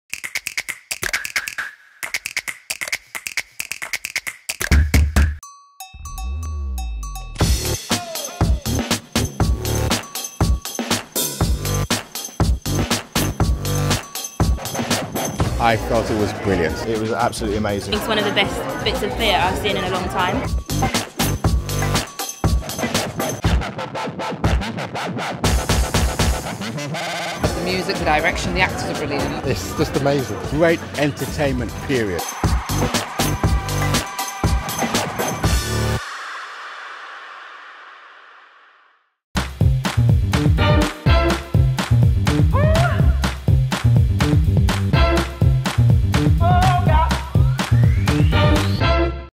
Barber Shop Chronicles Trailer
Following critically acclaimed seasons at the National Theatre, the West Yorkshire Playhouse and a world tour, Inua Ellams’ massively acclaimed Barber Shop Chronicles has arrived at the Roundhouse for the summer.

This show is also part of Kids Week - where for the whole of August, a child (aged 16 or under) can go to any participating show for free where tickets are available as long as they’re accompanied by a full paying adult!